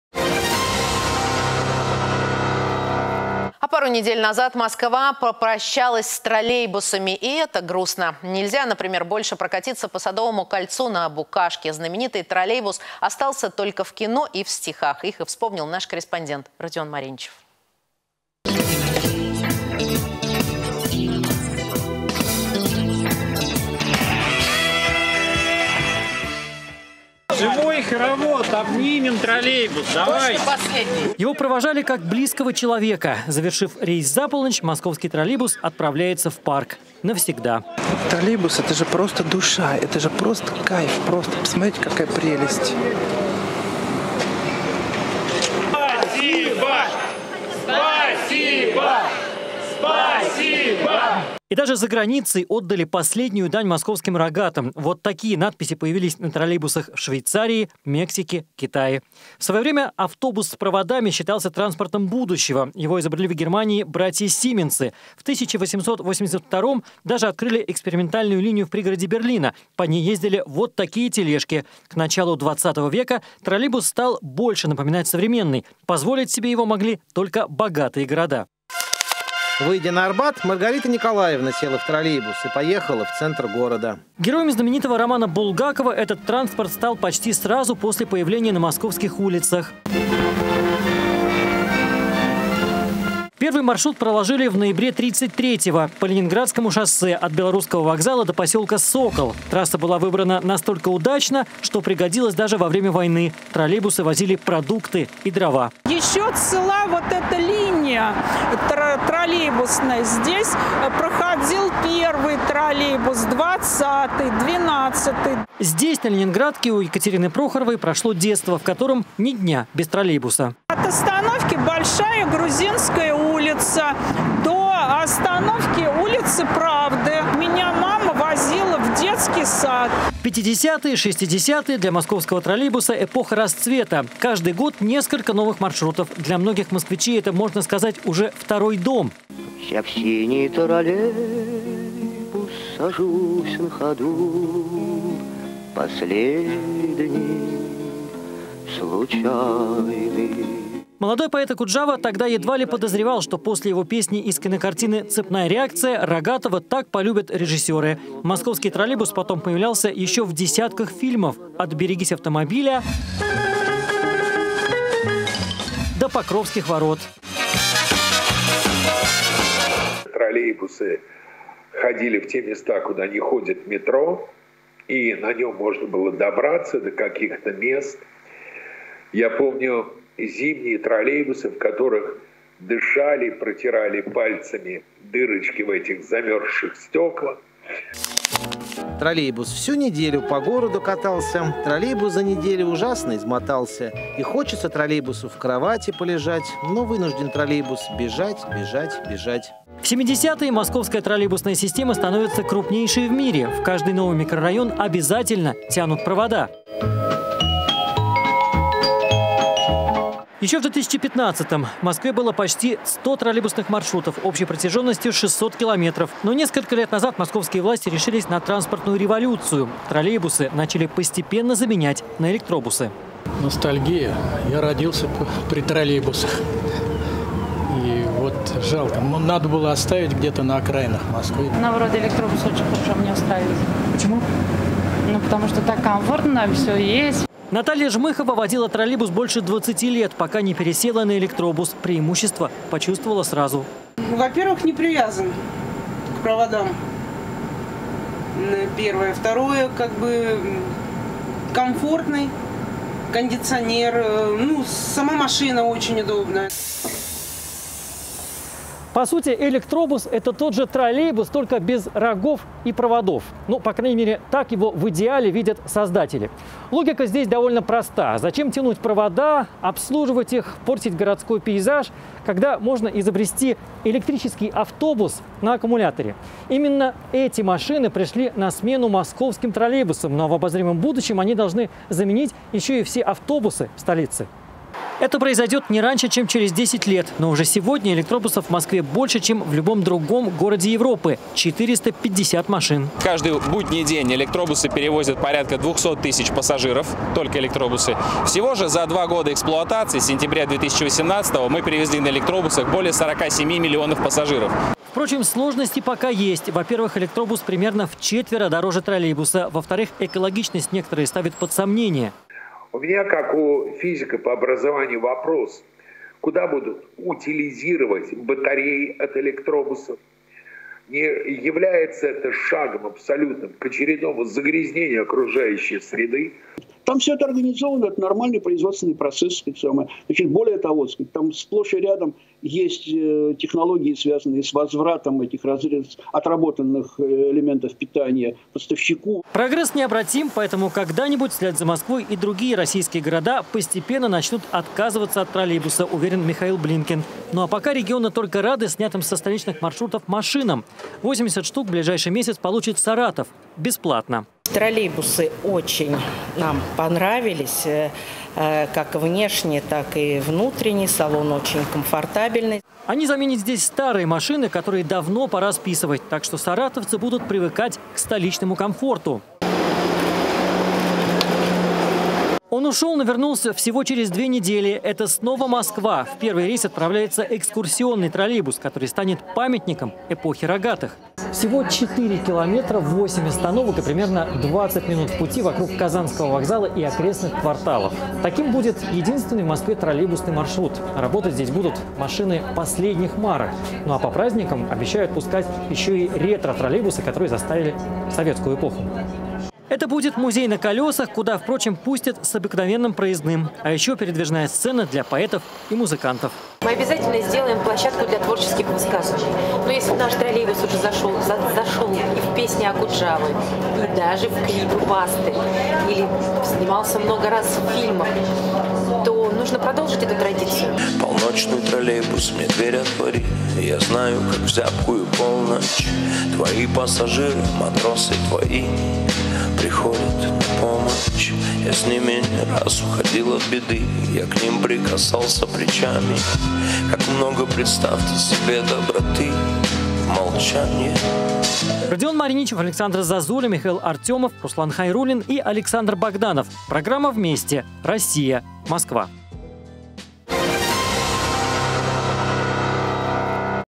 Москва простилась с троллейбусами
Пару недель назад Москва попрощалась с троллейбусами. И это грустно. Нельзя, например, больше прокатиться по Садовому кольцу на Букашке. Знаменитый троллейбус остался только в кино и в стихах. Их и вспомнил корреспондент «МИР 24» Родион Мариничев.